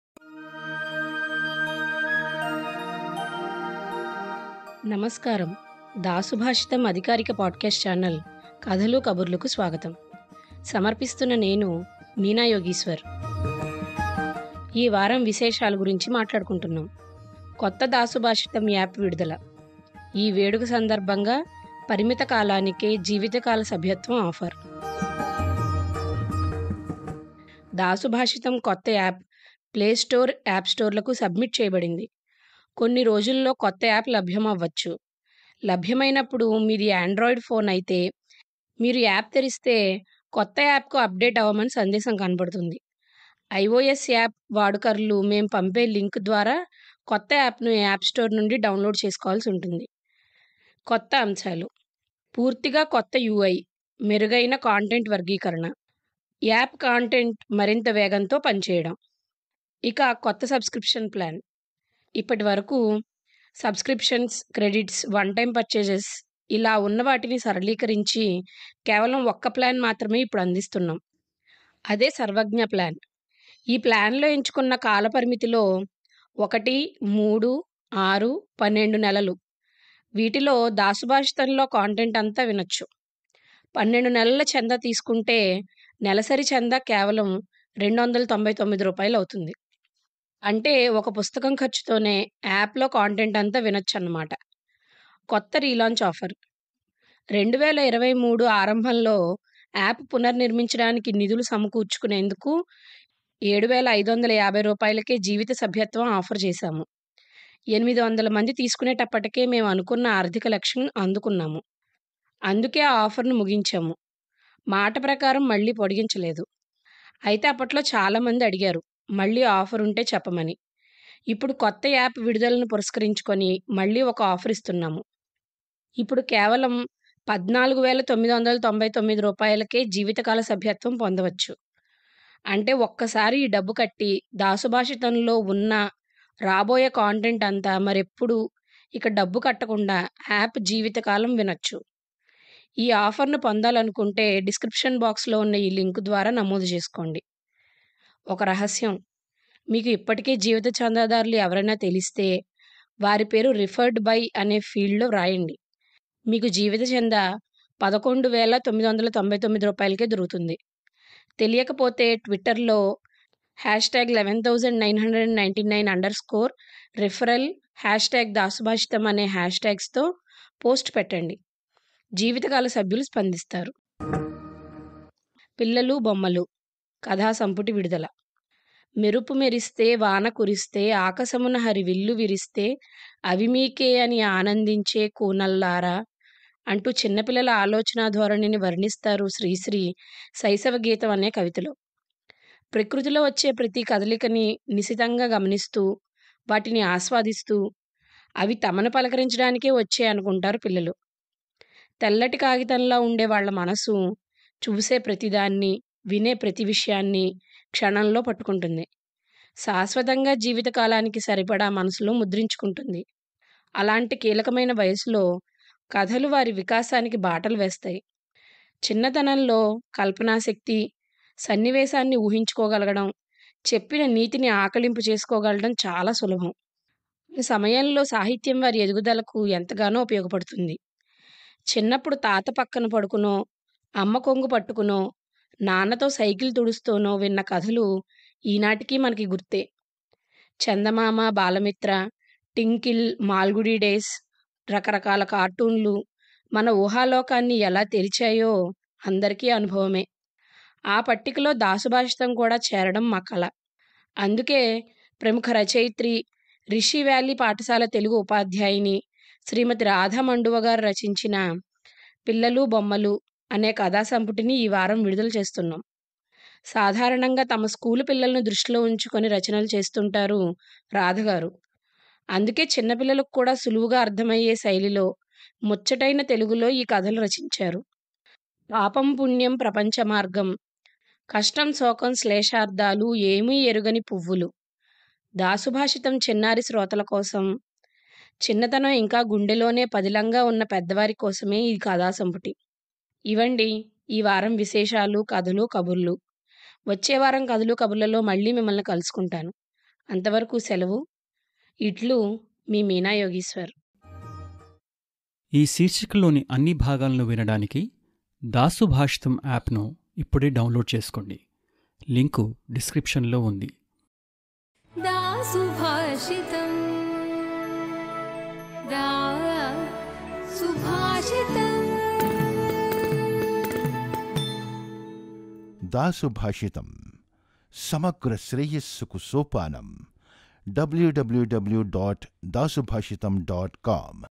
46. Kotta Dasubhashitam App Vidudala
Kathalu Kaburlu S02E46

మాది కంచుమర్రు అని అత్తిలి మండలంలో ఉన్న ఒక గ్రామం. ఎనిమిదేళ్ళ వయసు వరకూ నా బాల్యం అంతా ఆ ఊరిలోనే గడిచింది. పెంకుటిల్లు, వీధి అరుగులు, ఇంటి ఎదురుగా కాలువ, కాలువ గట్టు అంతా చెట్లు, ఇంటి ముందు కూడా రకరకాల మొక్కలు.. ఇలా కథల్లో వర్ణించే పల్లెటూరు వాతావరణంలో పెరిగాను నేను. అందుకే కథల్లో వచ్చే ఇలాంటి వర్ణనలను త్వరగా పట్టుకోగలను. ఆ ప్రదేశాలకు క్షణంలో చేరుకోగలను.

నా ఈ చిన్ని జీవితంలో అతిపెద్ద మలుపు మా నాన్నగారి బదిలీ. తాడేపల్లిగూడెం అనే పట్టణం వెళ్ళబోతున్నాం అని తెలిసినప్పుడు, నా ఊహ ఎంత గారడీ చేసిందో చెప్పలేను. అప్పటికి పట్టణం అంటే అత్తిలి, భీమవరమే తెలుసు నాకు. అది కూడా కొన్ని వీధులే. అందుకే నేను క్రమం తప్పకుండా చదివే కథల పుస్తకాన్ని ఆసరగా తీసుకున్నాను. అందులో జాతీయ, అంతర్జాతీయ కథలలో వర్ణించే పట్టణాలను ఊహించుకున్నాను.

విశాలమైన వీధికి అటు పక్క, ఇటు పక్క డాబాలు. వీధి చివర్లో రెండు అంతస్థుల మేడ. వారికి ఒక బొచ్చు కుక్క. వీధి మొగల్లో ఒక వైపు departmental store, మరో వైపు రోజాలు, డాలియాలు వంటి గొప్ప పువ్వులు అమ్మే boquet shop. రహదారికి రెండు వైపులా చెట్లు ఇలా ఉండేది నా ఊహ.

తీరా వెళ్ళాకా ఆ ఊహ కొంత మాత్రమే నిజం. అన్నీ పెంకుటిళ్ళే, రెండు వీధుల అవతల మా ఊళ్ళో ఉన్నదాని కన్నా కొంచెం పెద్ద కిరాణా కొట్టు ఉంది తప్ప, departmental stores గట్రా ఏమీ లేదు. ఇక boquet shop గురించి నేను చెప్పనవసరం లేదేమో. ఒక రెండు అంతస్థుల మేడ, చెట్లు మాత్రం ఉన్నాయి. చిన్న ఊరు నుండి, పెద్ద ఊరికి వచ్చినట్టు అనిపించింది మా వీధి చూసి.

ఈ విషయం గుర్తు చేసుకున్నప్పుడల్లా ఊహ అనేది చిన్నారి మనసులుకు, వారి ఎదుగుదలకు ఎంత ముఖ్యమా అనిపిస్తుంటుంది నాకు. నాకు నేనే ఒక అందమైన వీధిని సృష్టించుకుని, దాదాపు రెండు నెలలు ఆ వీధిలో మానసికంగా తిరగగలగాడినికి నా పఠనమే కదా కారణం. అందుకే మండువ రాధ గారు తన 'పిల్లలు - బొమ్మలు' పుస్తకానికి permission ఇచ్చినప్పుడు నా ద్వారా దాసుభాషితం చిన్నారి శ్రోతలకు ఈ పుస్తకం అందితే బాగుంటుంది అనిపించింది(పూర్తిగా నా స్వార్ధమే).

అందుకే, పాపం మీ శ్రవణ, మానసిక ఆరోగ్యాలను అస్సలు దృష్టిలో ఉంచుకోకుండా నా అద్భుతమైన గాత్రంలో రికార్డ్ చేసేహేసేశాను. ప్రభ గారు, మన content moniter చేసే రమా సుందరి గారు చాలా బాగా చదివారు అని మెచ్చుకున్నారు(privateగా లెండి) అంటే మీ ఆరోగ్యం safe అనే కదా. కాబట్టీ, నిరభ్యంతరంగా, నిర్భయంగా వినండి. మీకు బుజ్జాయిలు ఉంటే వారికీ వినిపించండి. పిల్లలు involve అయ్యారు కాబట్టీ శాపాలు గట్రా పెట్టను లెండి.

ఈ వారం newsletterలో ఈ release గురించే కాక, దాసుభాషితం app release గురించి కూడా update ఉంది. కాబట్టీ, కథలు-కబుర్లు వినక తప్పదు, blog post చదవకా తప్పదు.